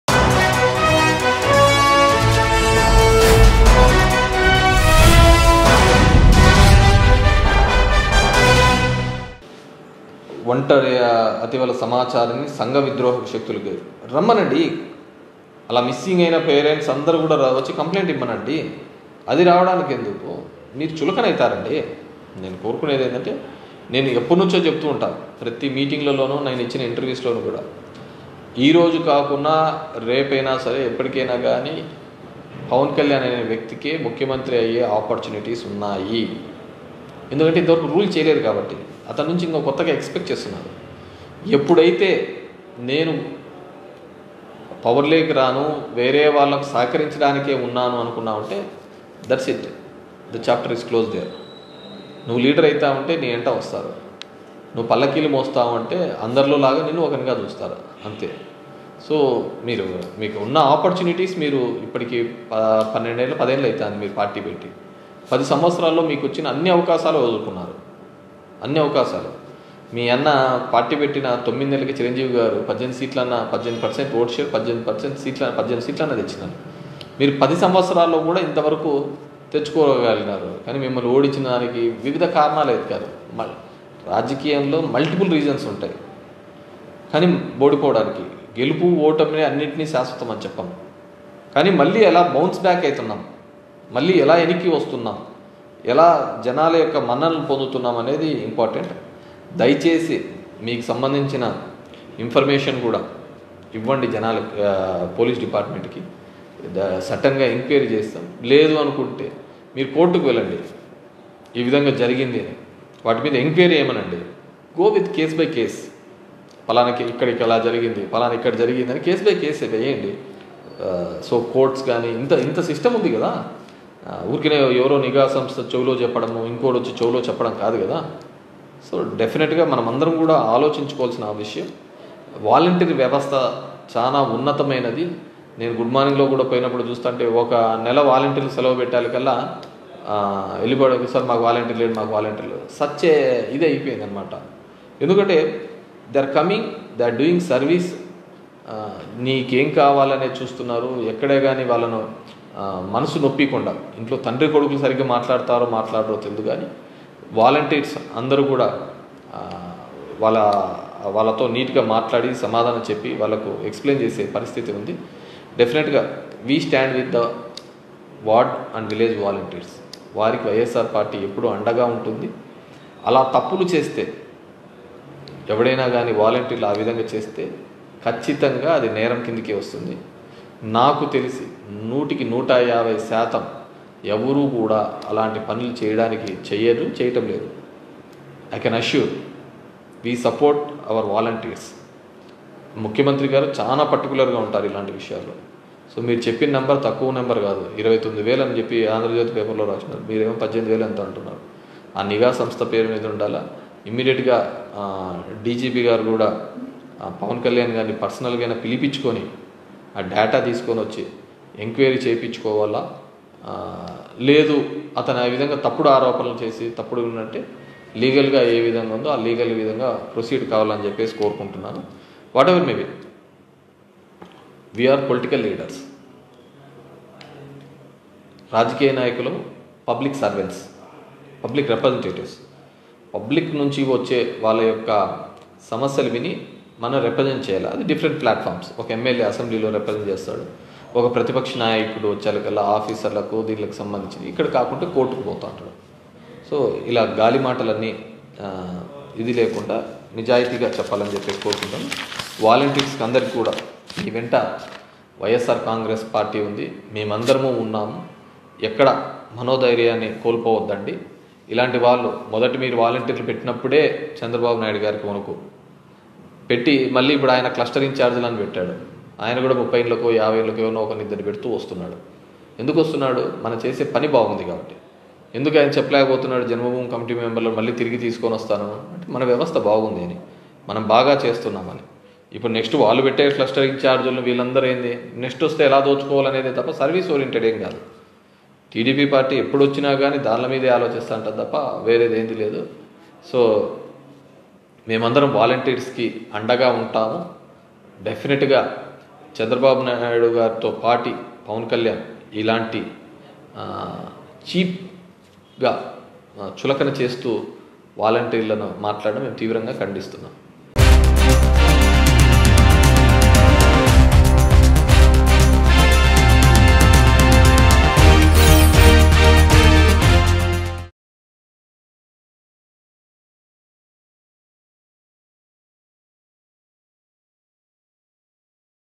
MLA Kethireddy Venkatarami Reddy Says Pawan Kalyan Will Become CM of Andhra Pradesh | Mango News
MLA Kethireddy Venkatarami Reddy Says Pawan Kalyan Will Become CM of Andhra Pradesh.